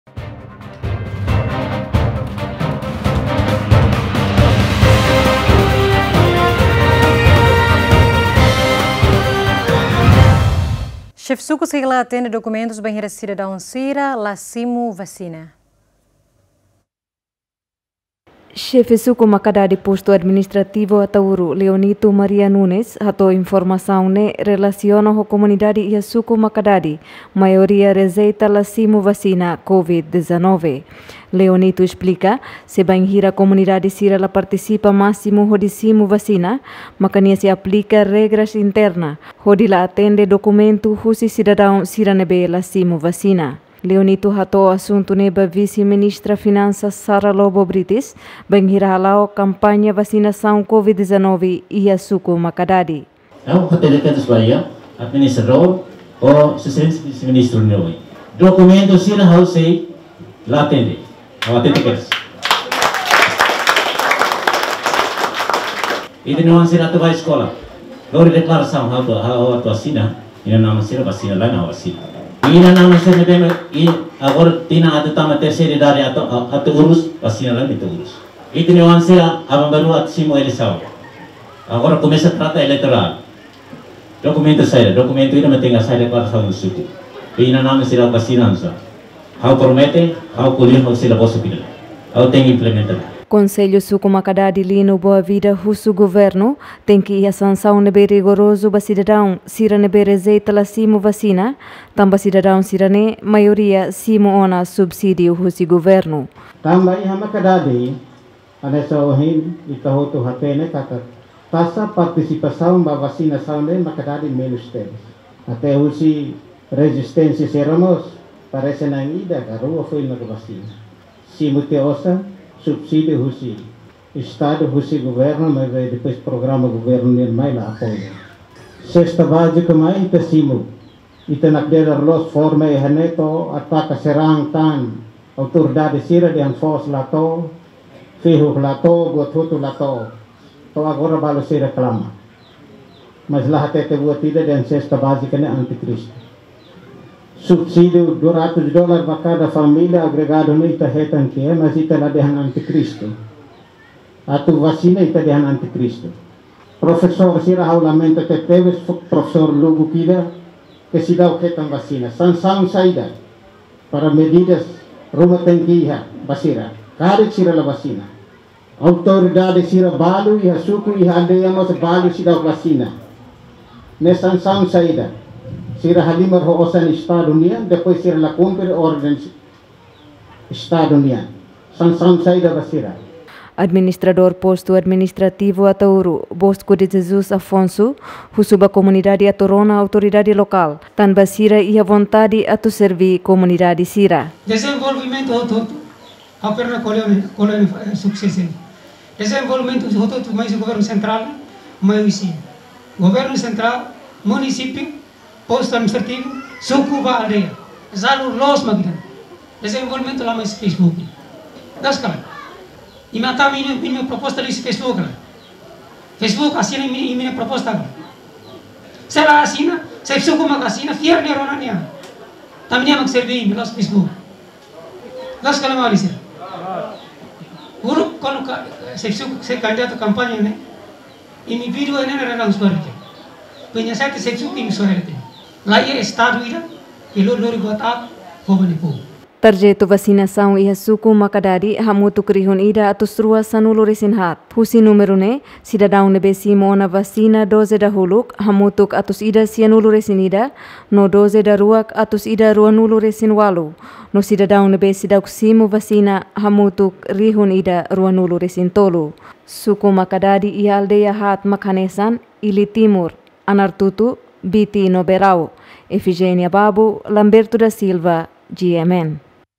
Sidadaun sira la simu vasina, Xefi Suku sei la atende dokumentus
Xefi Suku sei la atende dokumentus wainhira sidadaun sira la simu vasina